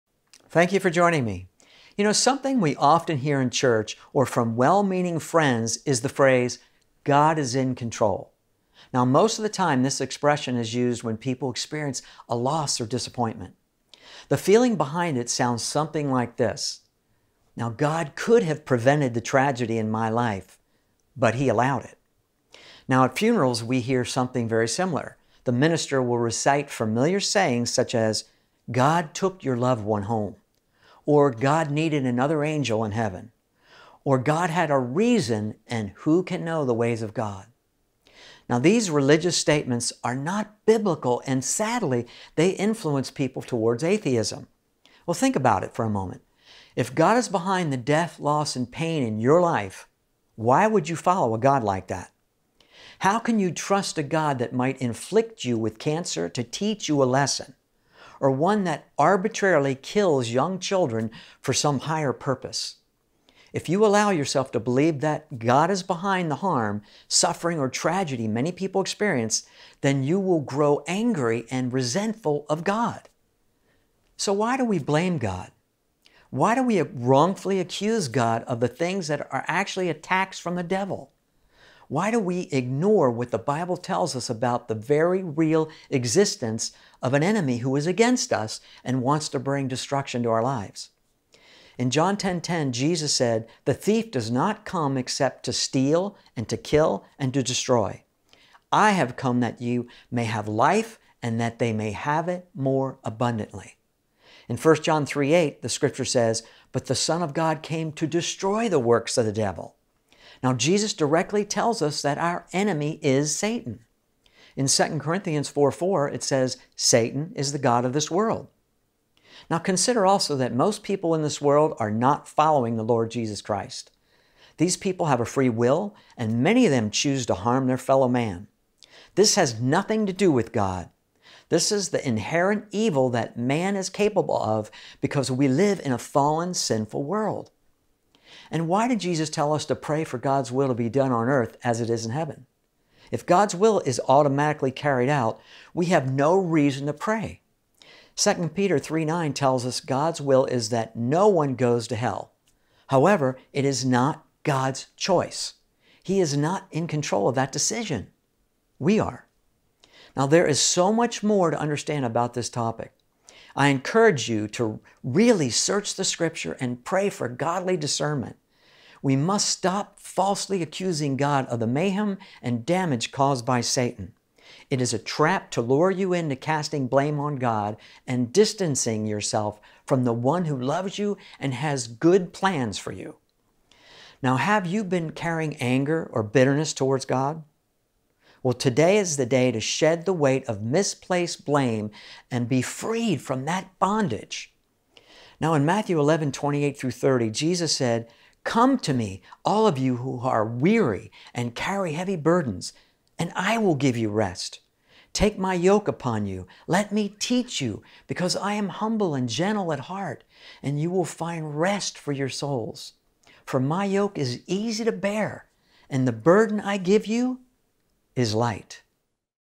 Where Was God? - Bill Wiese, "The Man Who Went To Hell" Author of "23 Minutes In Hell"
Where Was God?

Do you believe that God is behind the harm and suffering we see happening around us? Many people use the phrase, "God Is In Control" as a means of making God responsible for every loss, tragedy and disappointment we experience. Is God really the one orchestrating evil and hardship against mankind?

To support this ministry and help us reach people around the world